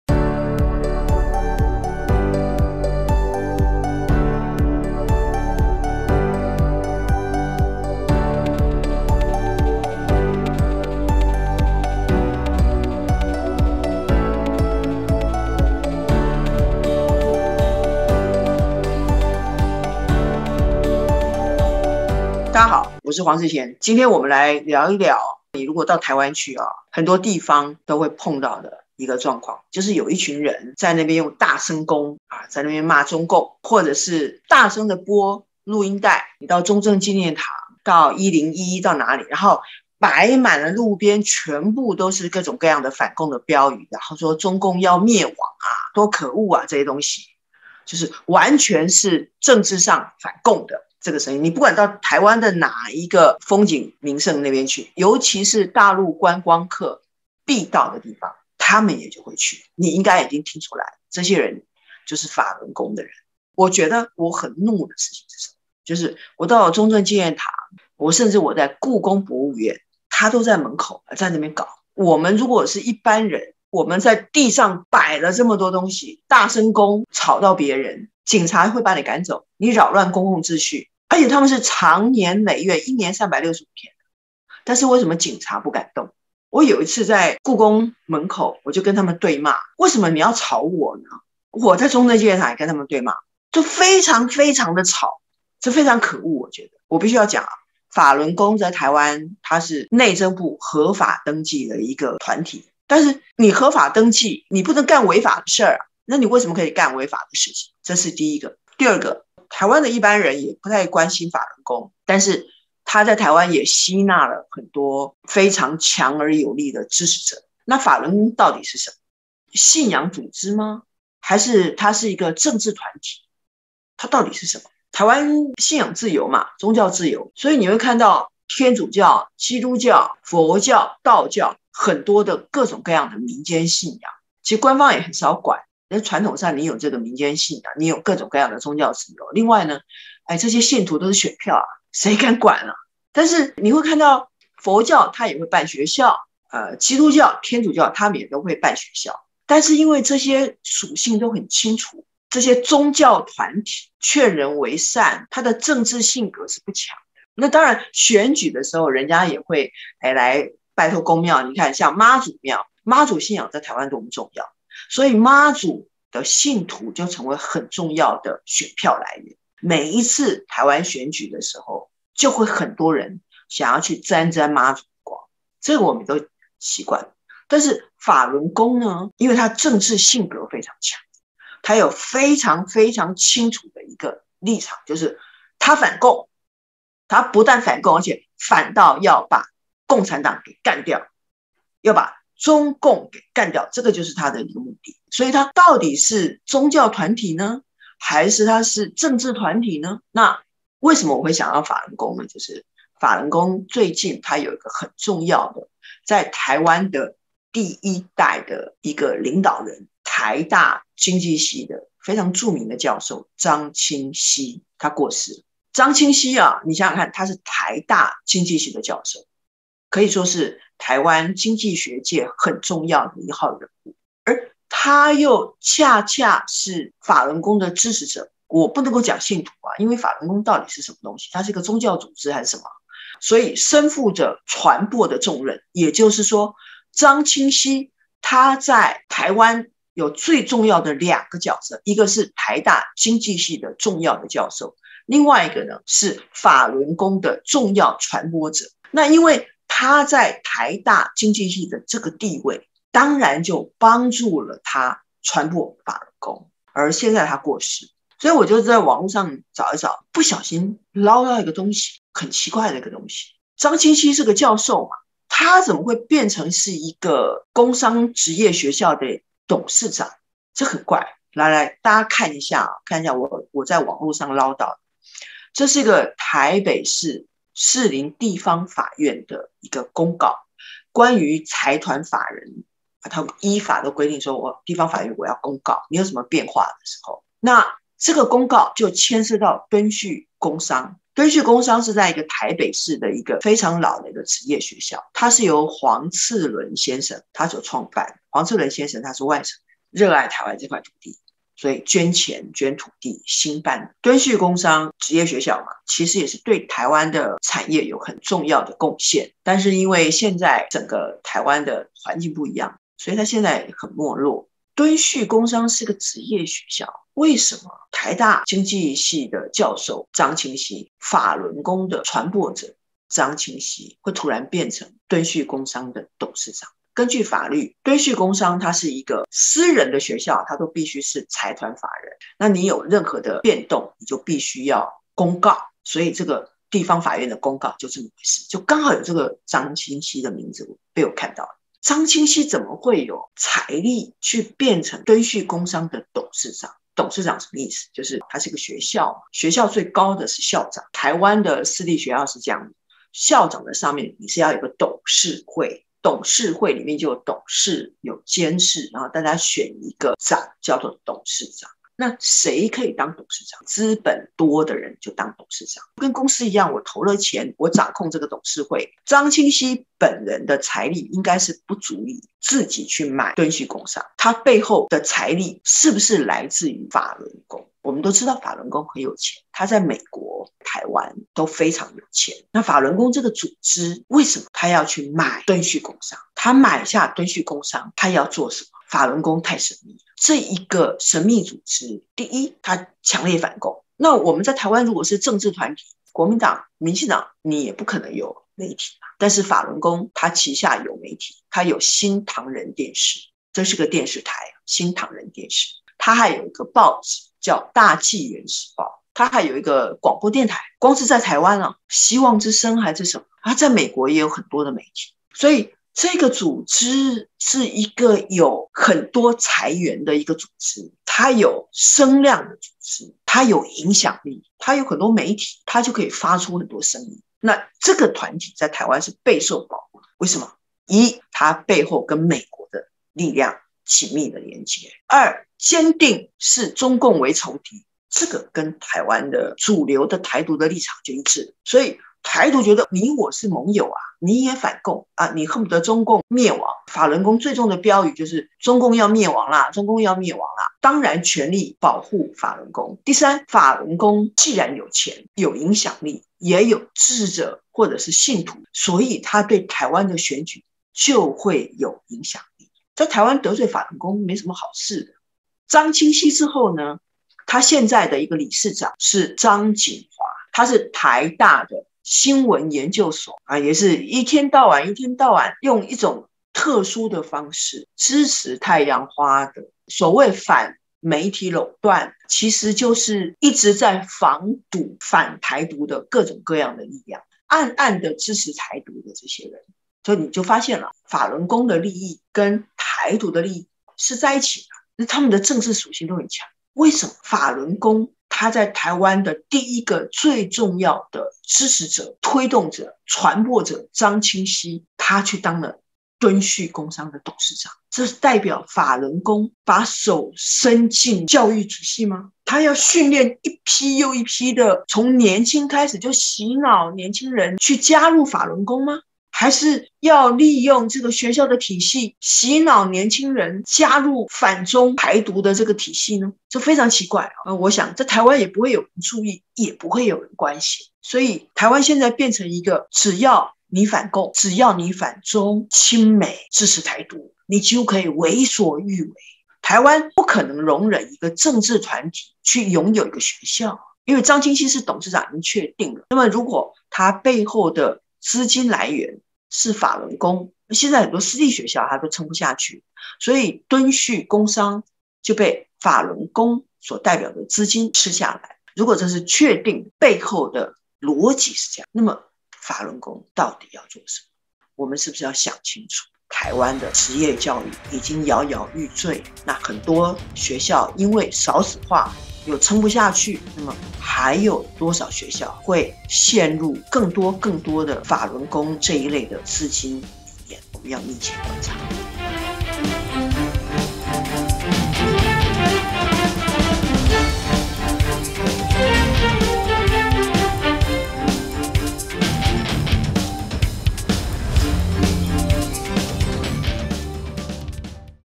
黃智賢夜問-政治意圖明顯的法輪功! 把手伸入教育界"鼓吹反中挺台獨"!?(主題版)
📌周一至周五 晚上8點
準時鎖定 YouTube:黃智賢夜問